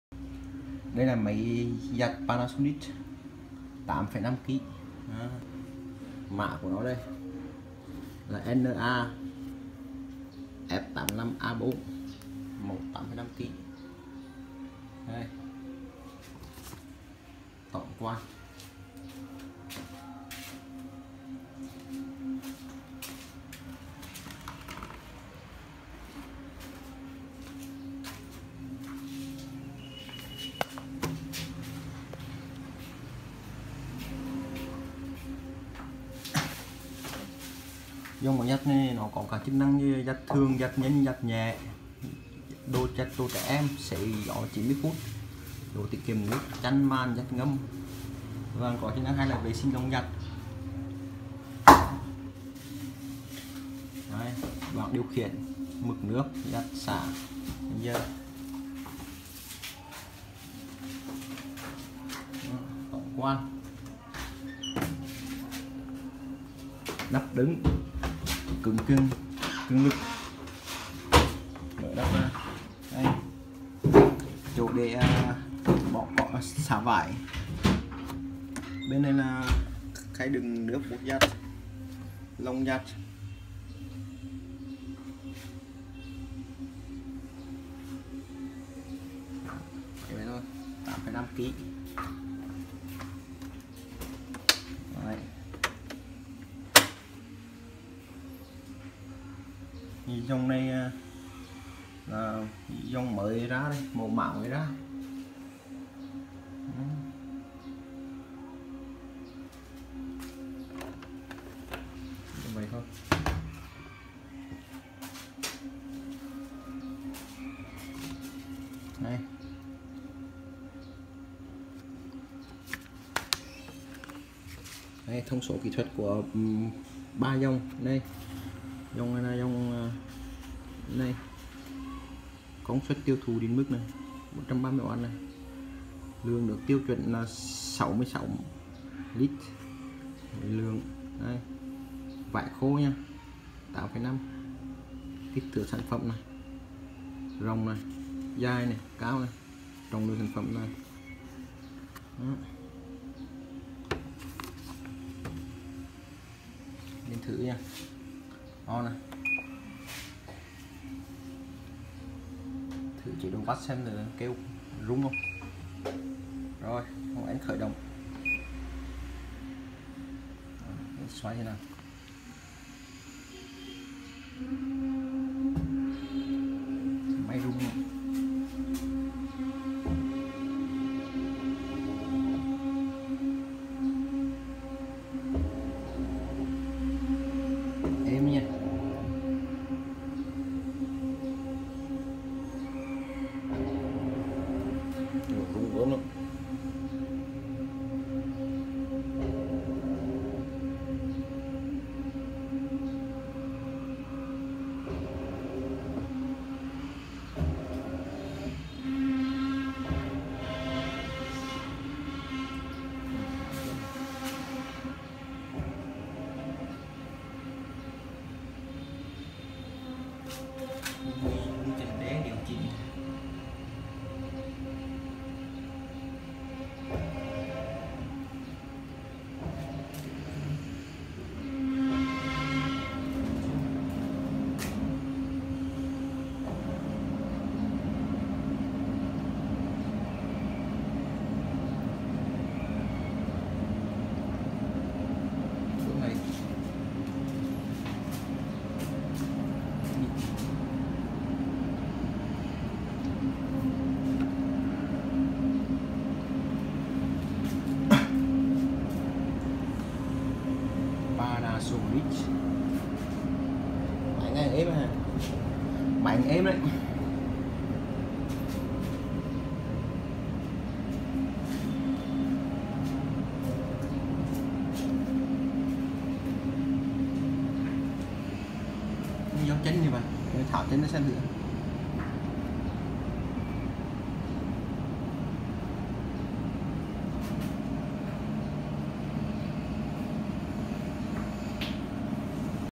Máy giặt Panasonic mua tại Điện Máy Xanh
Đây là máy giặt Panasonic tự động hoàn toàn, mẫu mới ra, mình đang trên tay máy giặt Panasonic mã Na F85A4 được mua tại điện máy xanh, ngoài ra còn có mã Na F90A4 và NA F100A4 nữa vì các mã này có thiết kế giống nhau chỉ khác nhau về khối lượng giặt tối đa mà thôi.